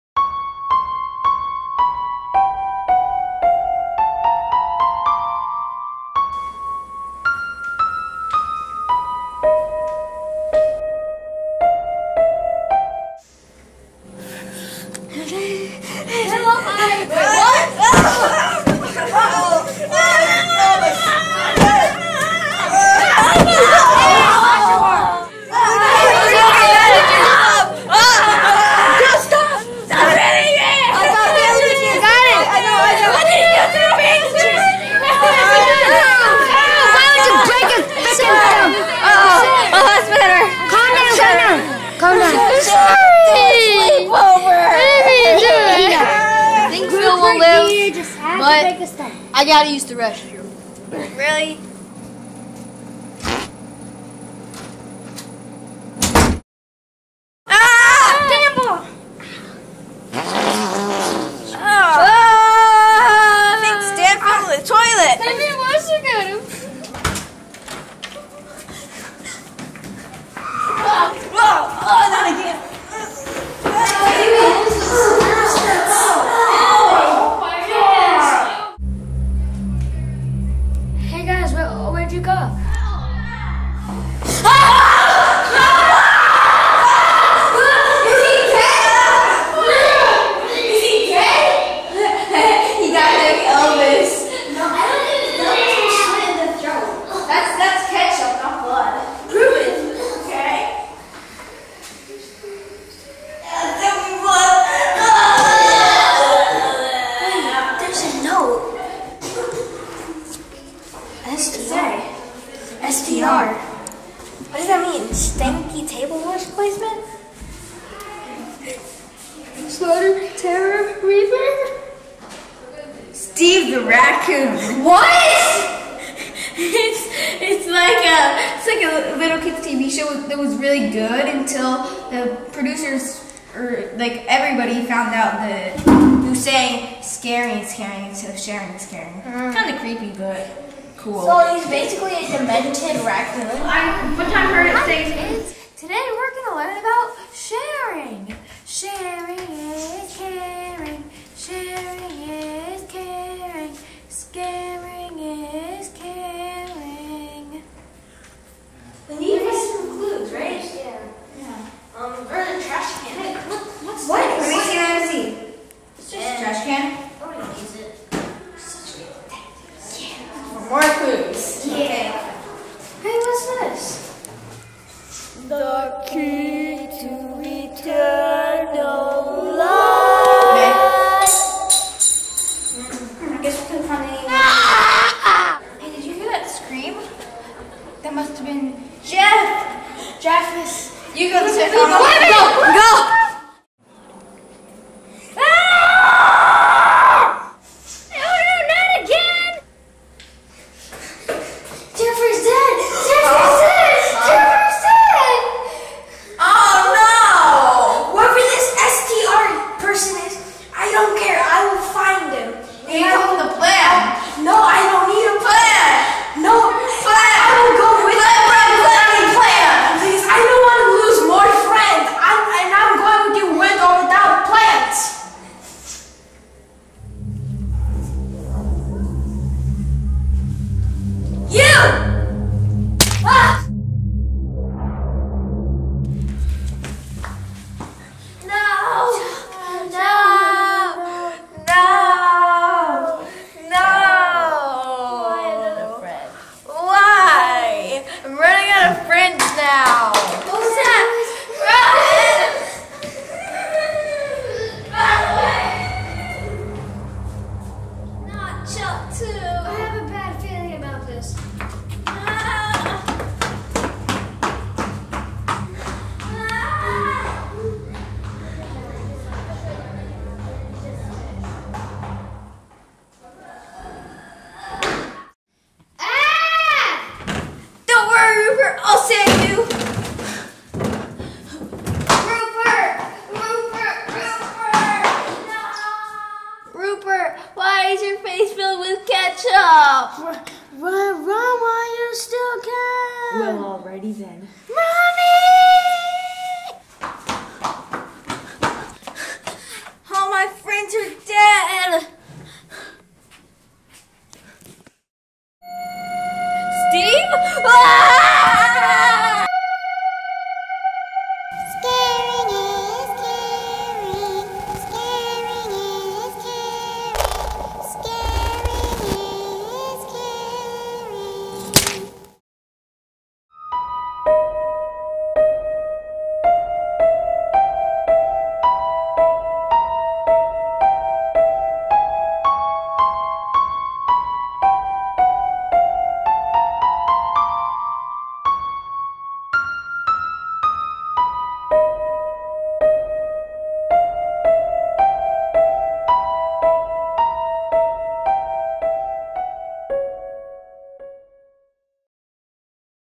THE KILLER TRASH CAN by Ed Studios & The Raccoon Squad
A killer trash can hunts down kids one by one.  Who is in the trash can? Will any of the boys survive?